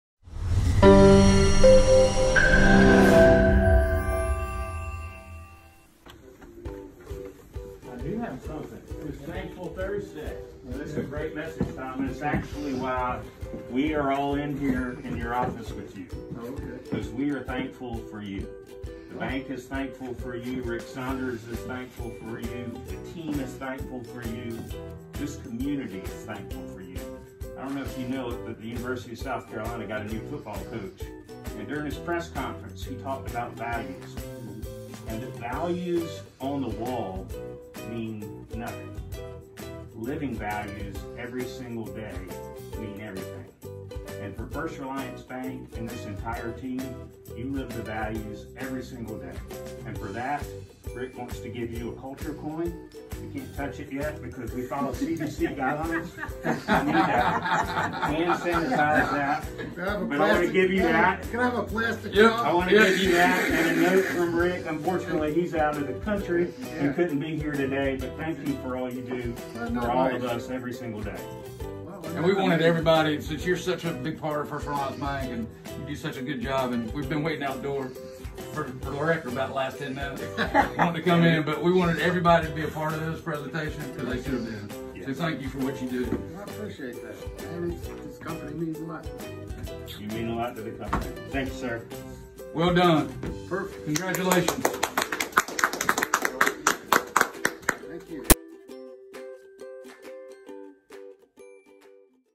Culture Coin Tom Ewart 3
I awarded Tom Ewart a Culture Coin as he has lived our bank's purpose to the highest degree possible.  Our community loves him, our customers love the service he provides, and the associates at the bank have deep respect for him.  He truly is one of our strong leaders in the bank.